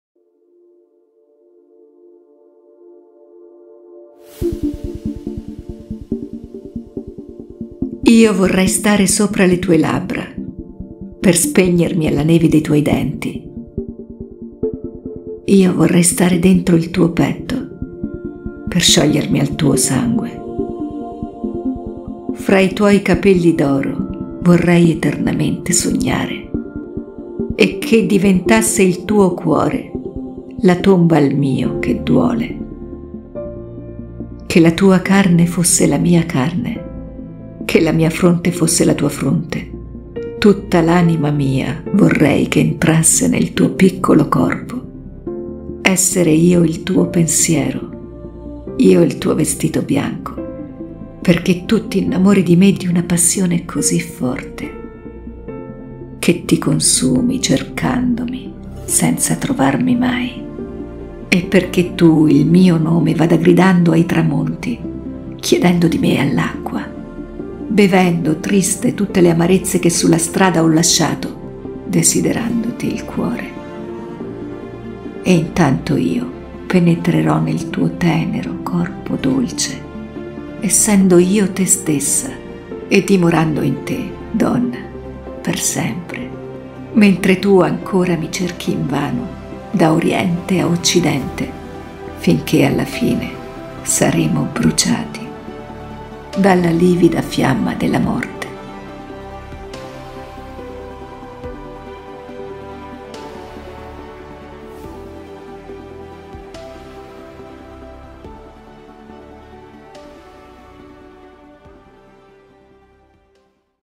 “Io vorrei stare sopra le tue labbra” poesia di Federico García Lorca, Legge Alessandra Paganelli
La lettura di questa poesia ci porta a riflettere sull'importanza della comunicazione e dell'espressione emotiva nelle nostre relazioni interpersonali, e sul potere delle parole nel trasmettere emozioni e pensieri profondi. Federico Garcia Lorca, con la sua maestria poetica, ci trasporta in un viaggio emotivo attraverso la sua poesia, lasciandoci con un profondo apprezzamento per la bellezza e la potenza delle parole.

Riprese e montaggio video: Giancarlo Ceci
Miniatura creata con Filmora AI